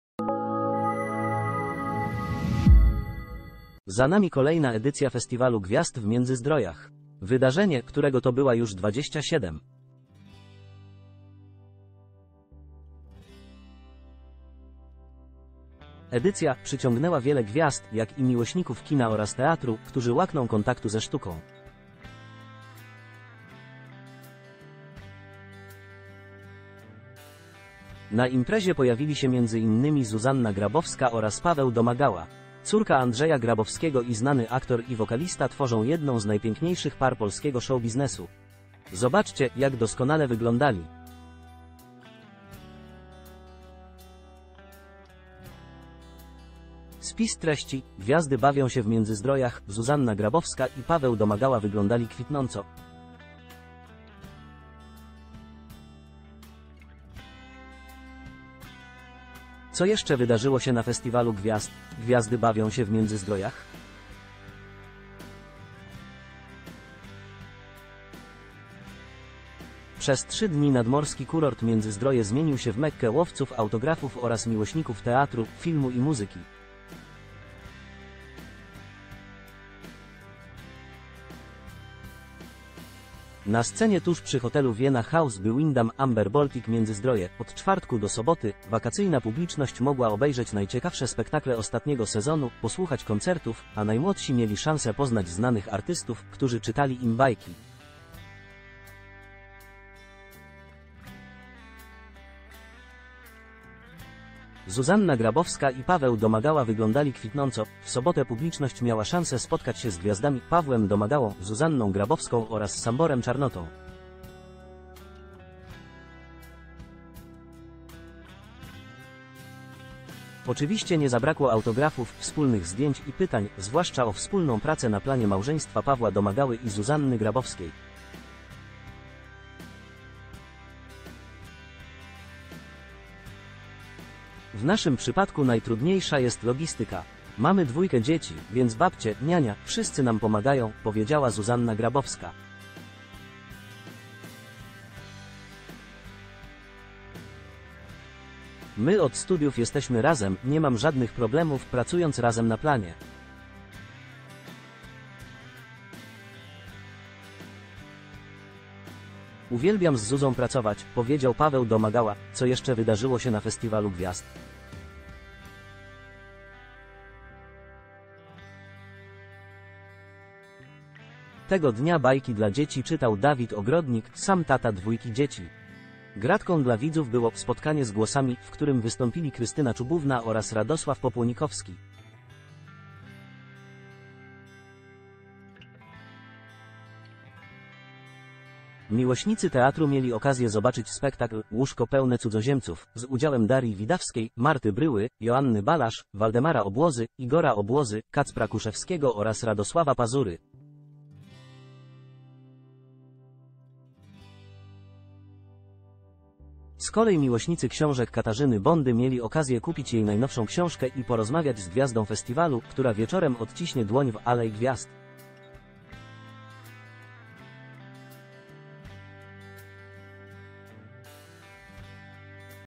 Zuzanna Grabowska w objęciach Pawła Domagały na Festiwalu Gwiazd w Międzyzdrojach! Najpiękniejsze ma
Za nami kolejna edycja Festiwalu Gwiazd w Międzyzdrojach. Wydarzenie, którego to była już 27. edycja, przyciągnęła wiele gwiazd, jak i miłośników kina oraz teatru, którzy łakną kontaktu ze sztuką. Na imprezie pojawili się m.in. Zuzanna Grabowska oraz Paweł Domagała. Córka Andrzeja Grabowskiego i znany aktor i wokalista tworzą jedną z najpiękniejszych par polskiego show-biznesu! Zobaczcie, jak doskonale wyglądali!
Spis treści

Gwiazdy bawią się w Międzyzdrojach!
Zuzanna Grabowska i Paweł Domagała  ...